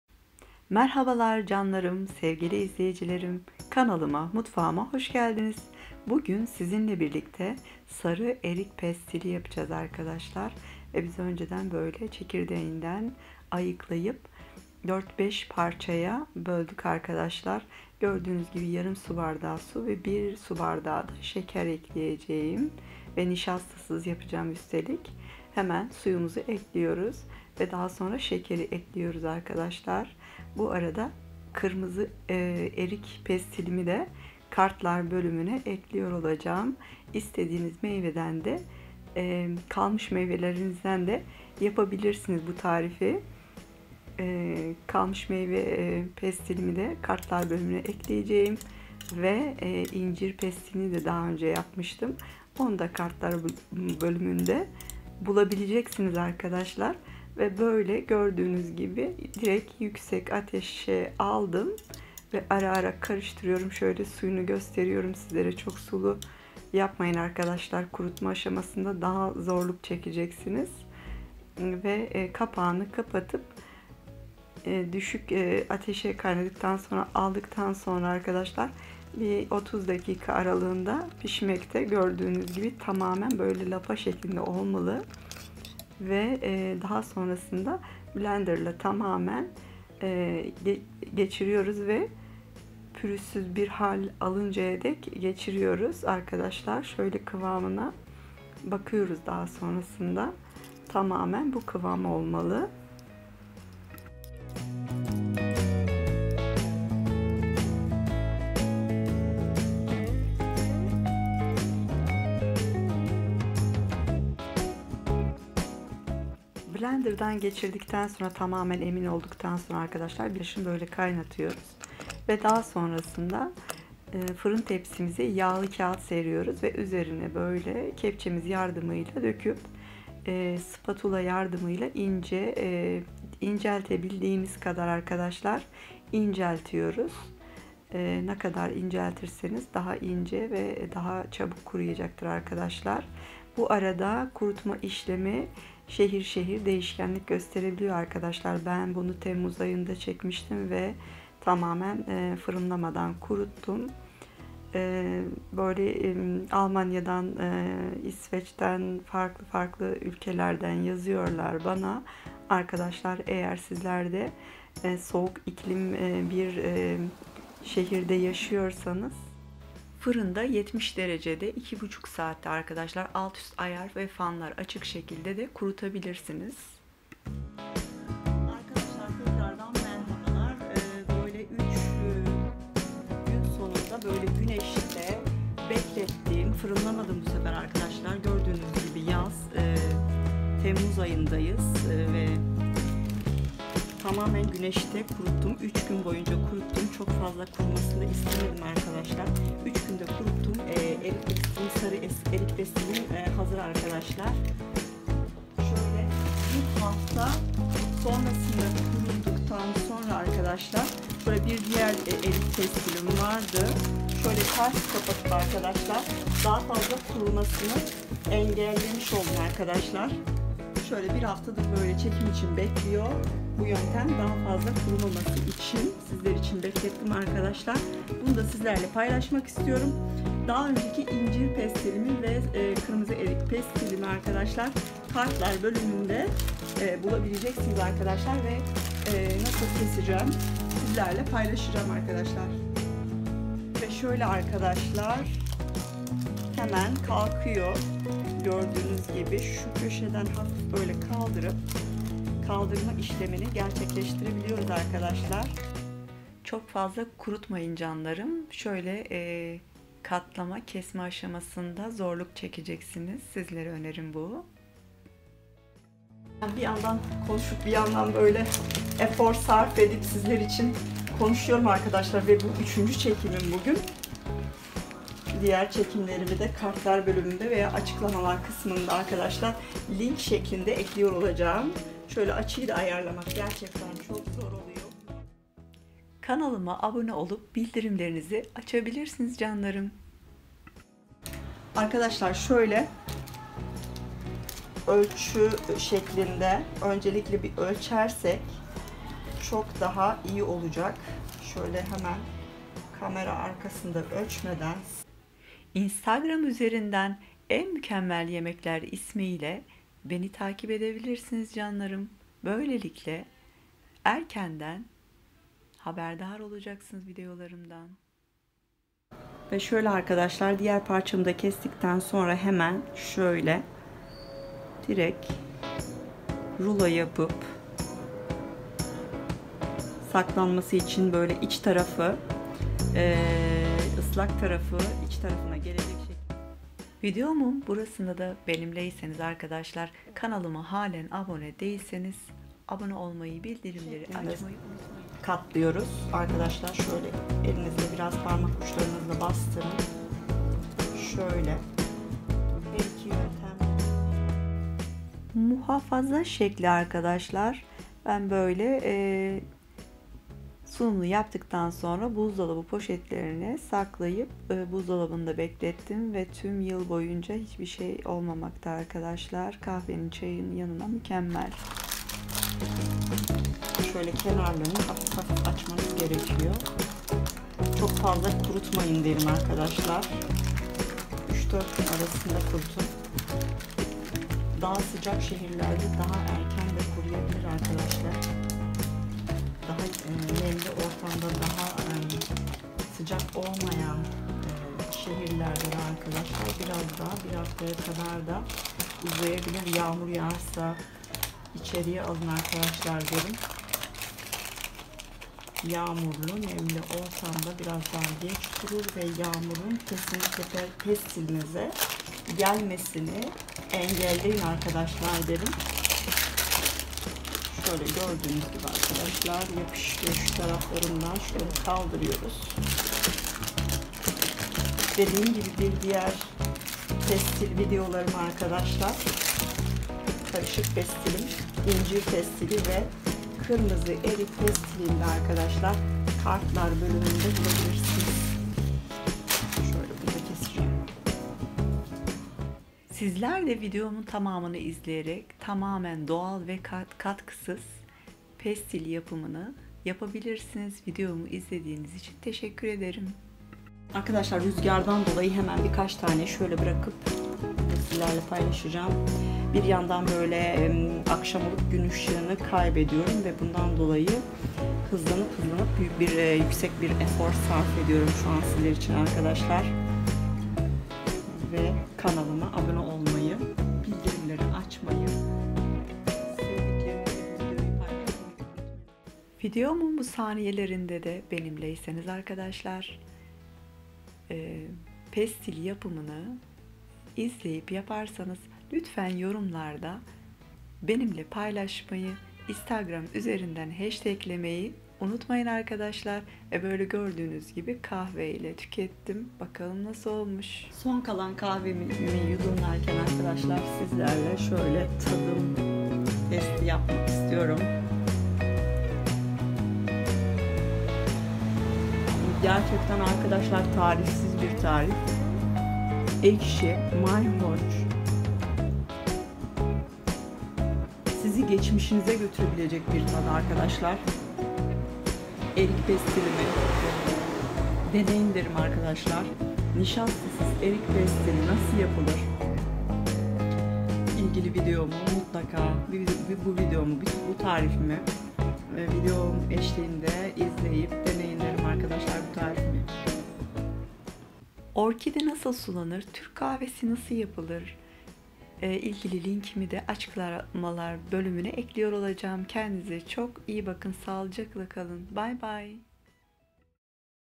Sarı Erik Pestili Nasıl Yapılır Üstelik Nişastasız - En Doğal Kolay Erik PESTİLİ Yapımı YemekTarifi
En doğalından kolay anlaşılır üstelik nişastasız yaptım nişastalı yapınca canlarım dişe yapışıyor yerken bu tarif daha doğal üstelik ağızda kötü tad bırakmıyor nişastalı biraz bırakabilir bilginize canım abonelerim...🌹erikpestilinasılyapılır